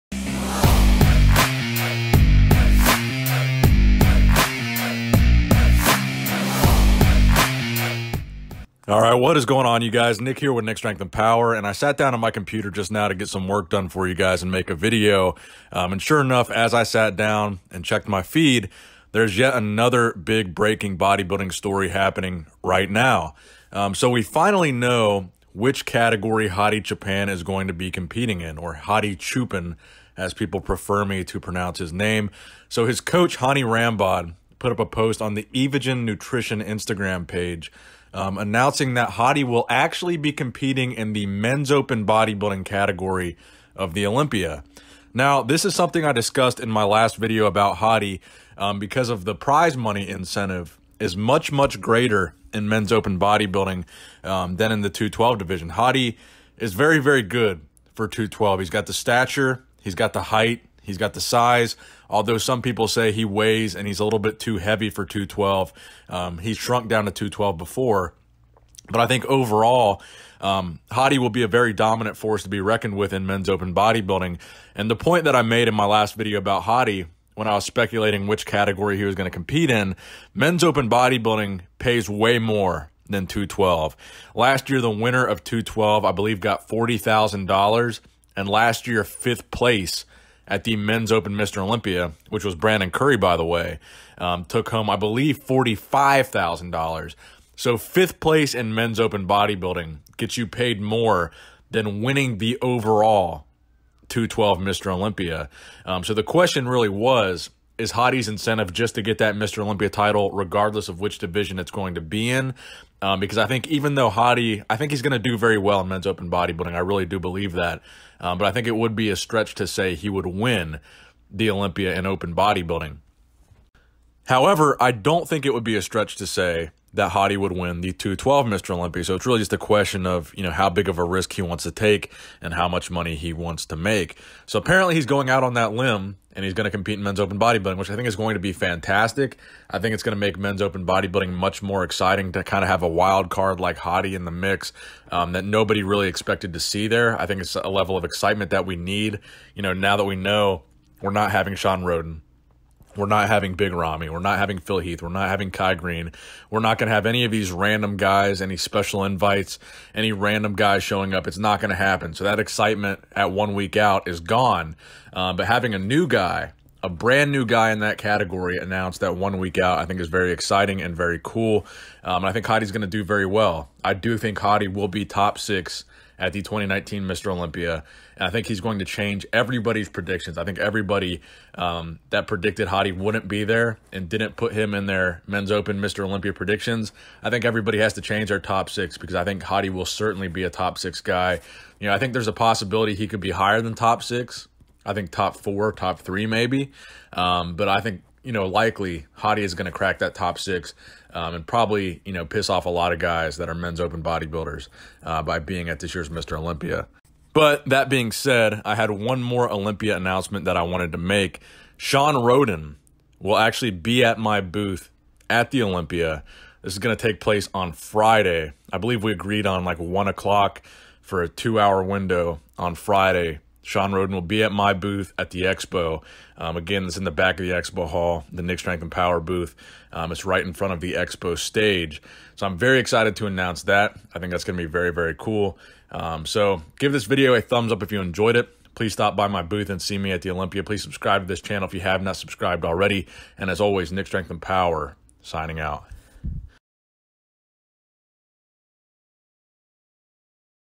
Hadi Choopan Will Compete in OPEN Bodybuilding at Olympia
Hadi Choopan makes official  Olympia Announcement.
*Interact with me on social media*